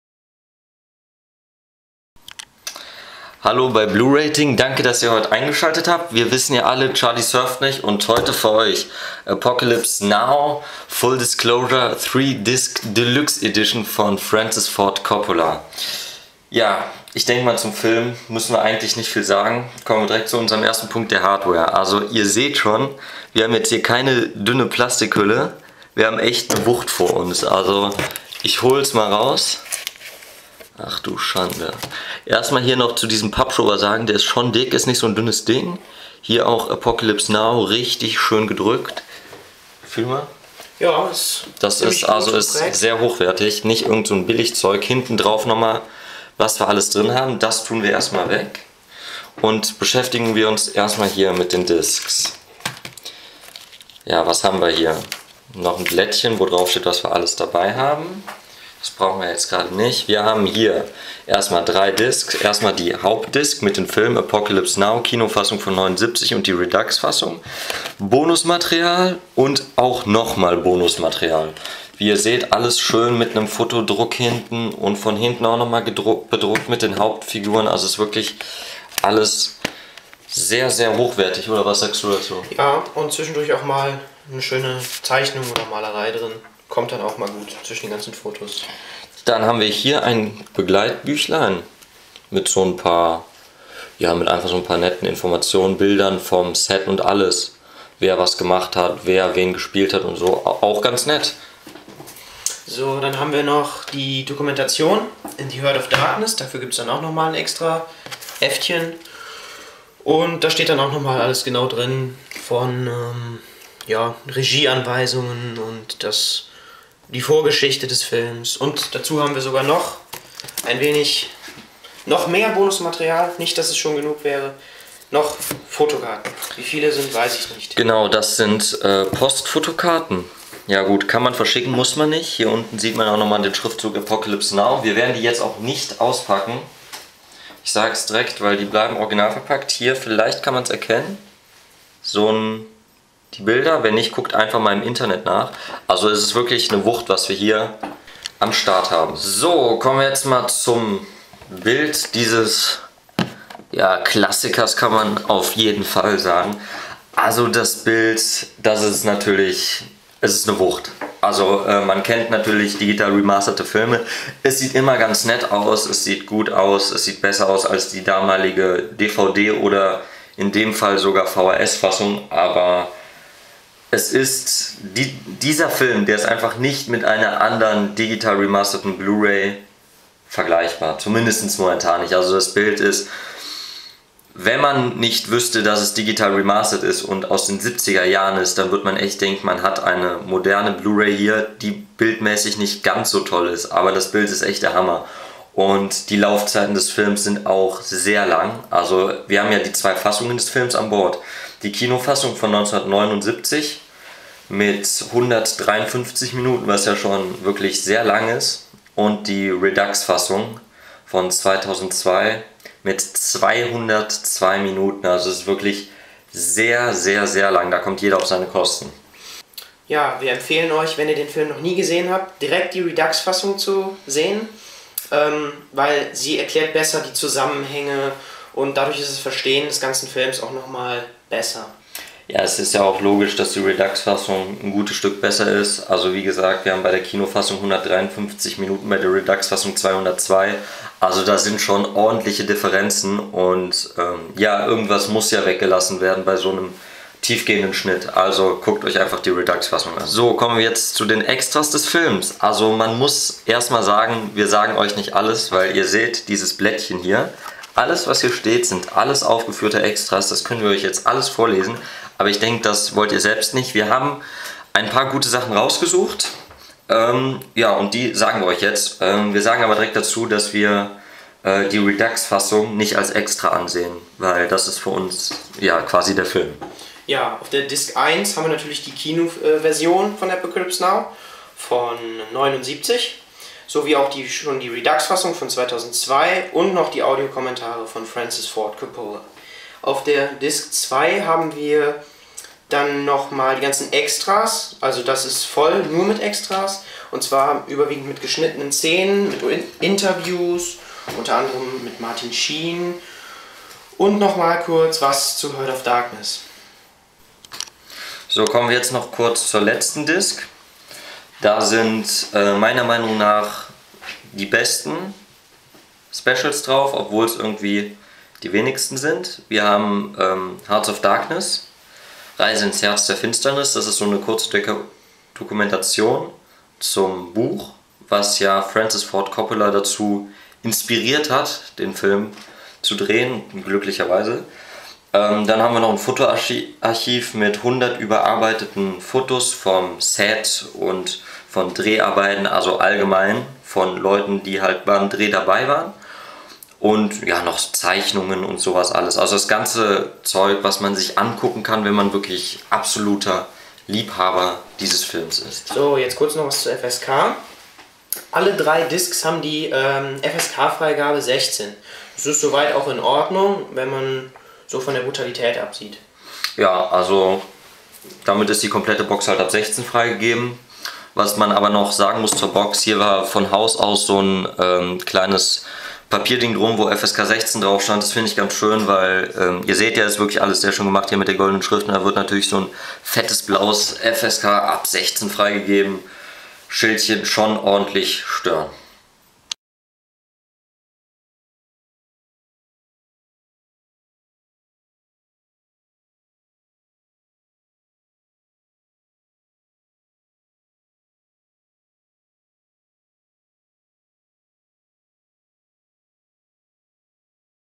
Apocalypse Now - Full Disclosure Deluxe Edition Review
Bei den Specials hat man sich bei der Sprachauswahl "Deutsch" verschrieben!
Der Link mit dem Inhalt:
Sprachen: Redux Fassung
Deutsch DTS-HD MA 5.1
Englisch DTS-HD MA 5.1
Untertitel:
Deutsch, Englisch; Deutsch für Hörgeschädigte
Spieldauer:
Kinofassung: 153 Minuten
Redux-Version: 202 Minuten
Bildformat(e):
1920x1080p (2.35:1)